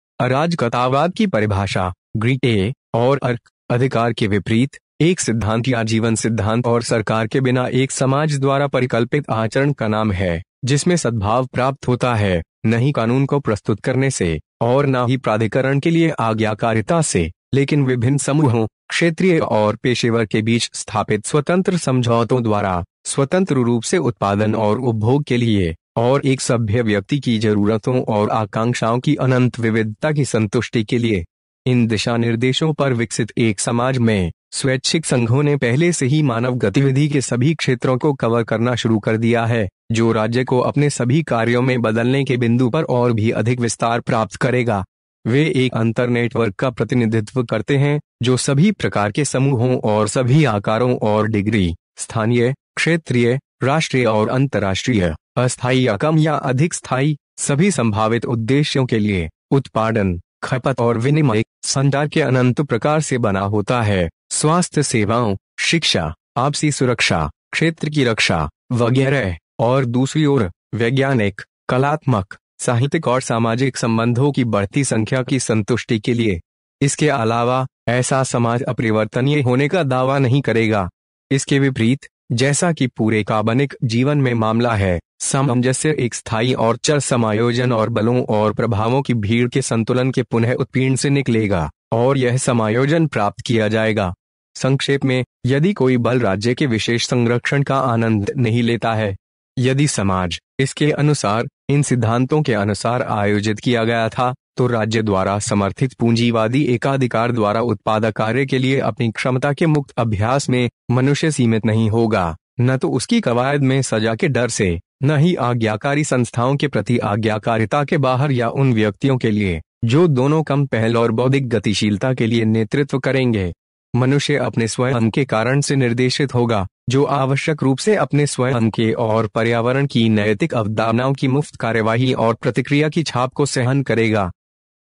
(En indi) अराजकतावाद क्या है? अराजकतावाद की परिभाषा
अराजकतावाद की परिभाषा (ग्रीक ए-, और अर्क, अधिकार के विपरीत), एक सिद्धांत या जीवन सिद्धांत और सरकार के बिना एक समाज द्वारा परिकल्पित आचरण का नाम है, जिसमें सद्भाव प्राप्त होता है, नहीं  कानून को प्रस्तुत करने से, और न ही प्राधिकरण के लिए आज्ञाकारिता से, लेकिन विभिन्न समूहों, क्षेत्रीय और पेशेवर के बीच स्थापित स्वतंत्र समझौतों द्वारा, स्वतंत्र रूप से उत्पादन और उपभोग के लिए, और एक सभ्य व्यक्ति की जरूरतों और आकांक्षाओं की अनंत विविधता की संतुष्टि के लिए।

   इन दिशानिर्देशों पर विकसित एक समाज में, स्वैच्छिक संघों ने पहले से ही मानव गतिविधि के सभी क्षेत्रों को कवर करना शुरू कर दिया है, जो राज्य को अपने सभी कार्यों में बदलने के बिंदु पर और भी अधिक विस्तार प्राप्त करेगा।  वे एक अंतर नेटवर्क का प्रतिनिधित्व करते हैं, जो सभी प्रकार के समूहों और सभी आकारों और डिग्री, स्थानीय, क्षेत्रीय, राष्ट्रीय और अंतर्राष्ट्रीय, अस्थायी या कम या अधिक स्थायी, सभी संभावित उद्देश्यों के लिए: उत्पादन, खपत और विनिमय, संचार के अनंत प्रकार से बना होता है।  , स्वास्थ्य सेवाओं, शिक्षा, आपसी सुरक्षा, क्षेत्र की रक्षा, वगैरह;  और दूसरी ओर, वैज्ञानिक, कलात्मक, साहित्यिक और सामाजिक संबंधों की बढ़ती संख्या की संतुष्टि के लिए।

   इसके अलावा, ऐसा समाज अपरिवर्तनीय होने का दावा नहीं करेगा।  इसके विपरीत, जैसा कि पूरे कार्बनिक जीवन में मामला है, सामंजस्य एक स्थायी और चर समायोजन और बलों और प्रभावों की भीड़ के संतुलन के पुनः उत्पीड़न से निकलेगा, और यह समायोजन प्राप्त किया जाएगा।  संक्षेप में, यदि कोई बल राज्य के विशेष संरक्षण का आनंद नहीं लेता है।

   यदि समाज, इसके अनुसार, इन सिद्धांतों के अनुसार आयोजित किया गया था, तो राज्य द्वारा समर्थित पूंजीवादी एकाधिकार द्वारा उत्पादक कार्य के लिए अपनी क्षमता के मुक्त अभ्यास में, मनुष्य सीमित नहीं होगा;  न तो उसकी कवायद में सजा के डर से, न ही आज्ञाकारी संस्थाओं के प्रति आज्ञाकारिता के बाहर या उन व्यक्तियों के लिए, जो दोनों कम पहल और बौद्धिक गतिशीलता के लिए नेतृत्व करते हैं।  मनुष्य अपने स्वयं के कारण से निर्देशित होगा, जो आवश्यक रूप से अपने स्वयं के और पर्यावरण की नैतिक अवधारणाओं की मुफ्त कार्रवाई और प्रतिक्रिया की छाप को सहन करेगा।